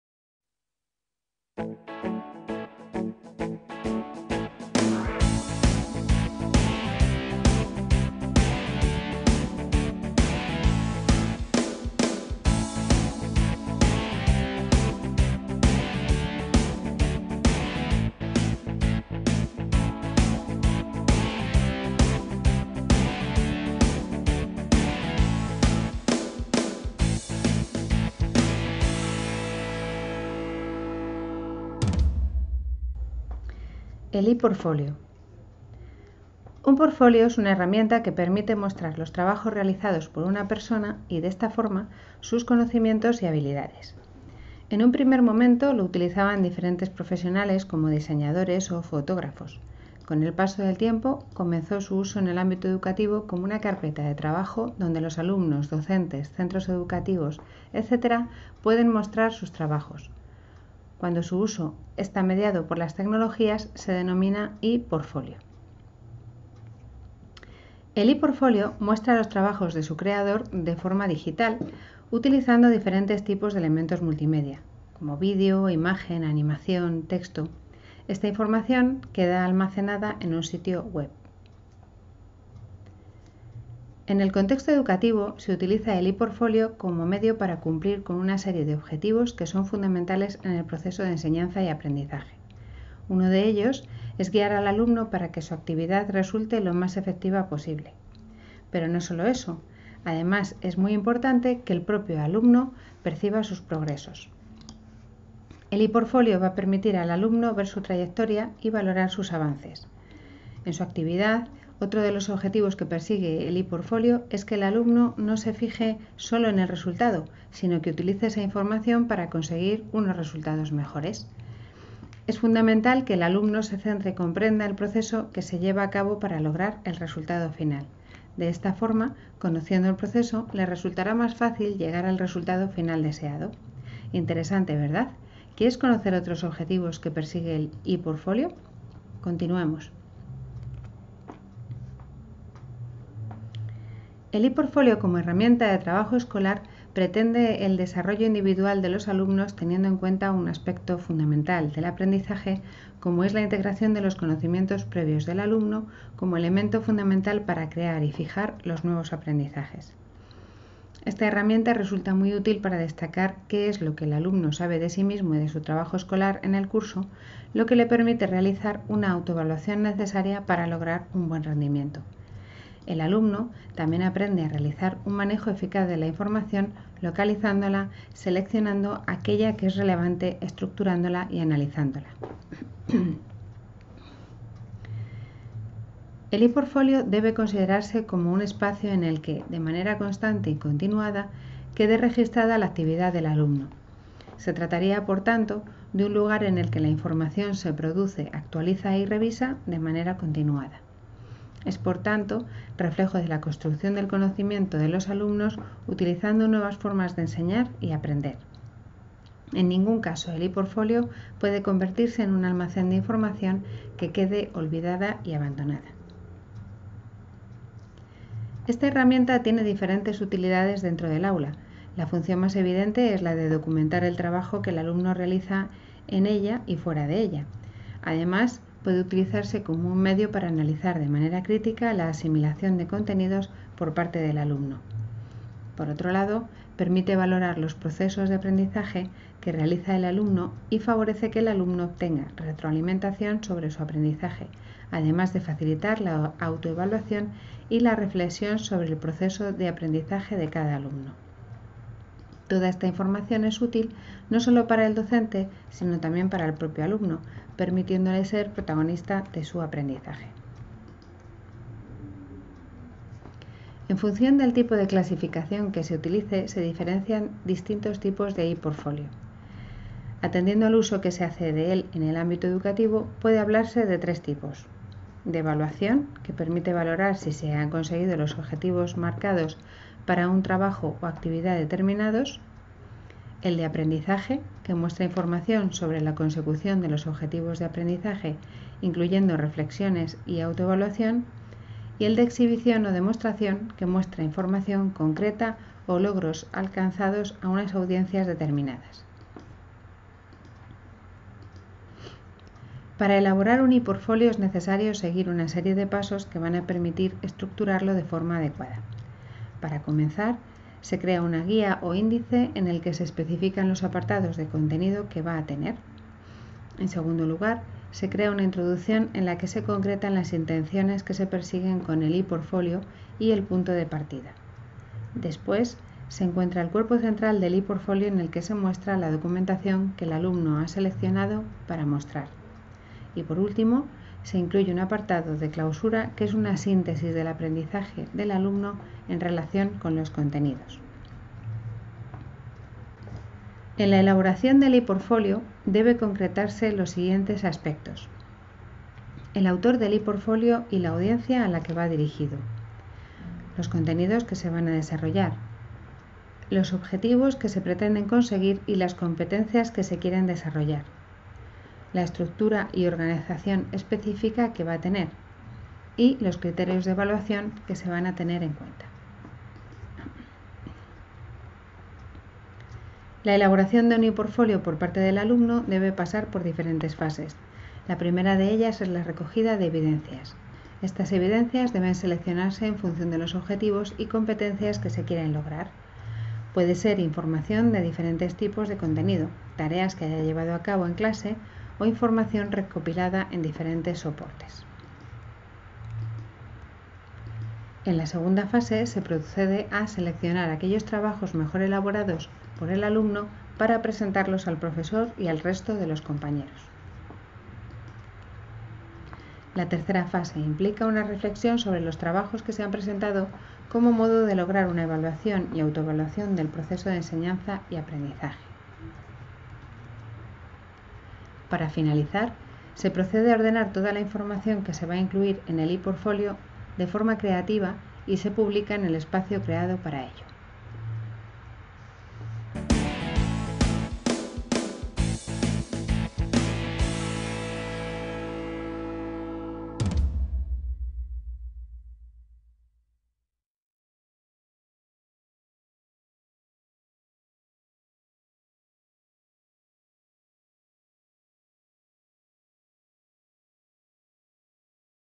El ePortfolio
El ePortfolio es una herramienta muy útil para utilizarla en la evaluación de tus alumnos. No hay un cambio real de la metodología sino cambia también la evaluación